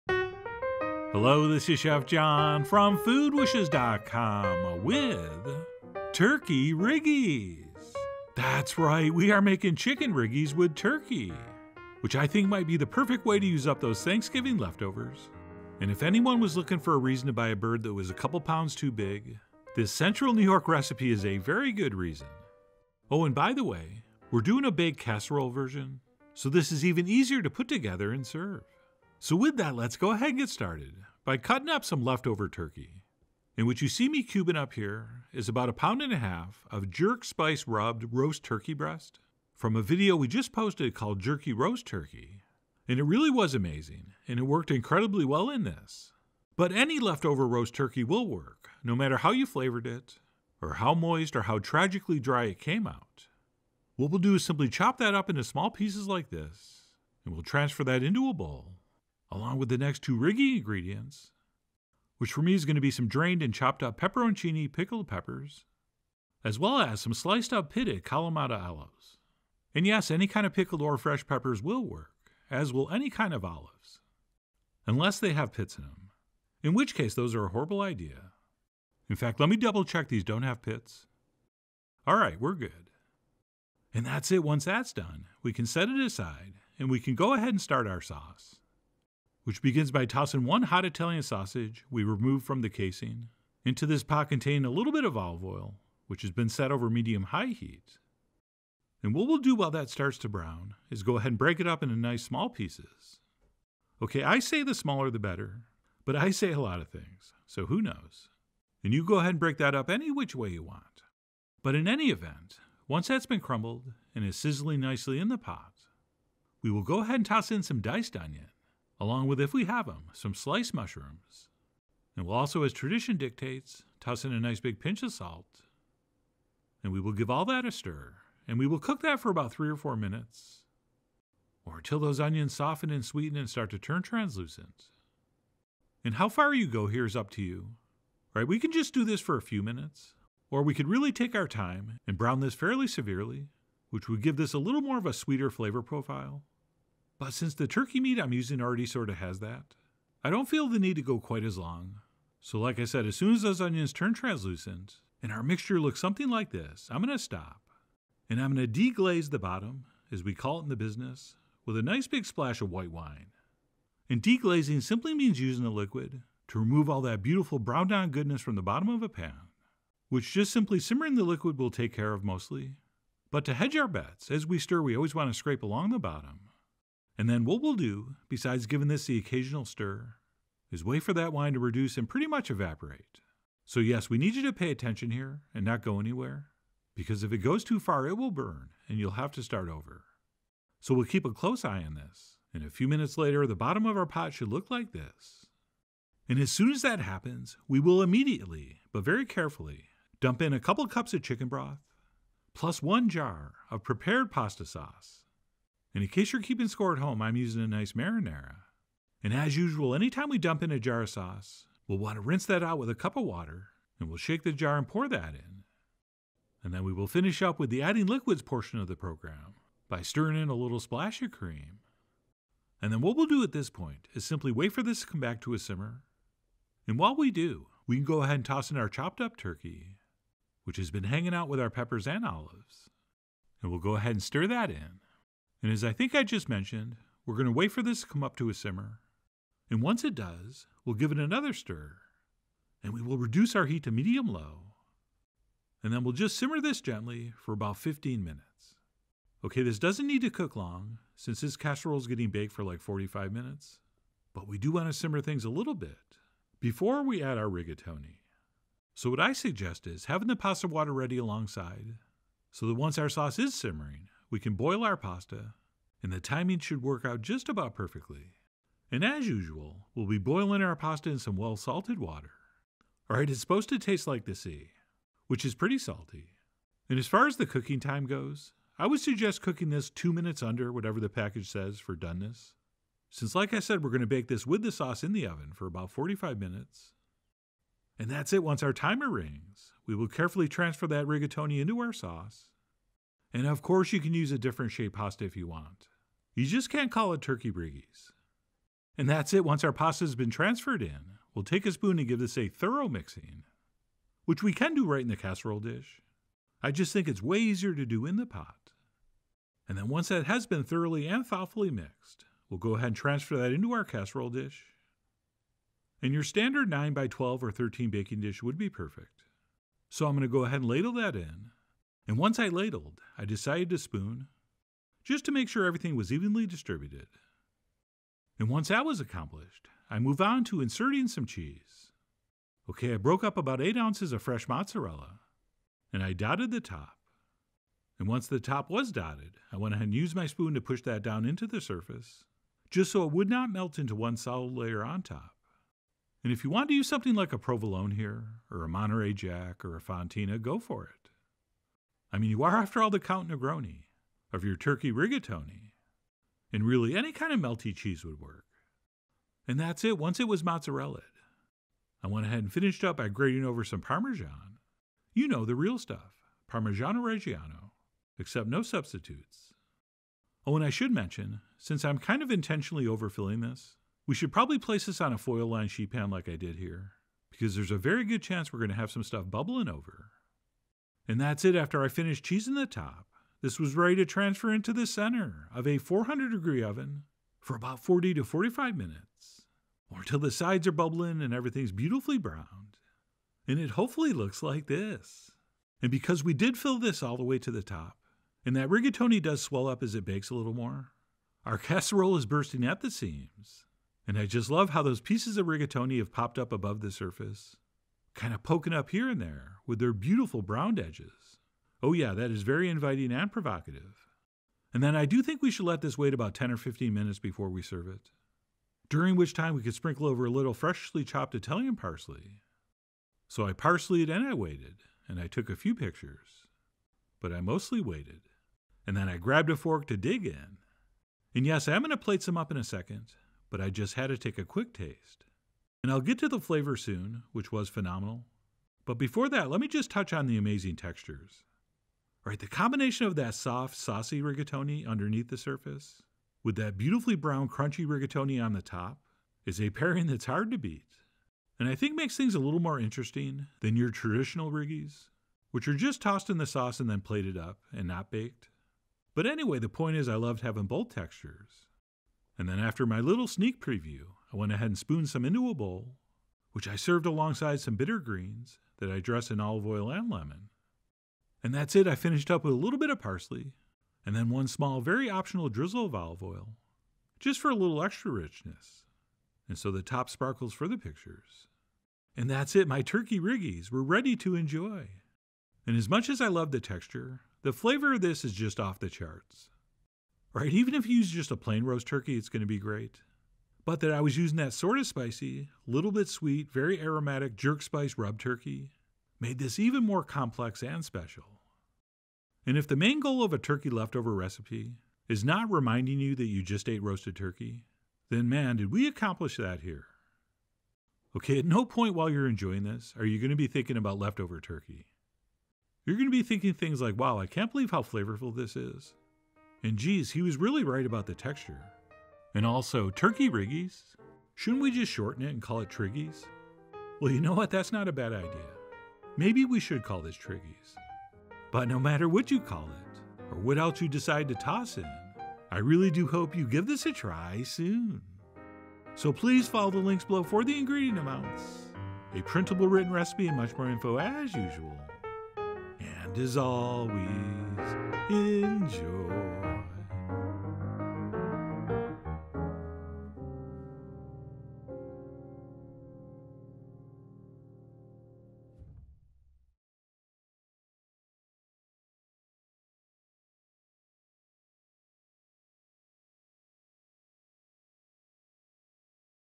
Turkey Riggies - Baked Rigatoni Turkey Casserole - Food Wishes
In case you’re not familiar with “riggies,” it’s a Central New York specialty featuring rigatoni pasta tossed with chicken and peppers in a spicy tomato sauce, and when baked into a casserole, it’s a perfect way to use up that leftover Thanksgiving turkey. Enjoy!

To become a Member of Food Wishes, and read Chef John’s in-depth article about Turkey Riggies, follow this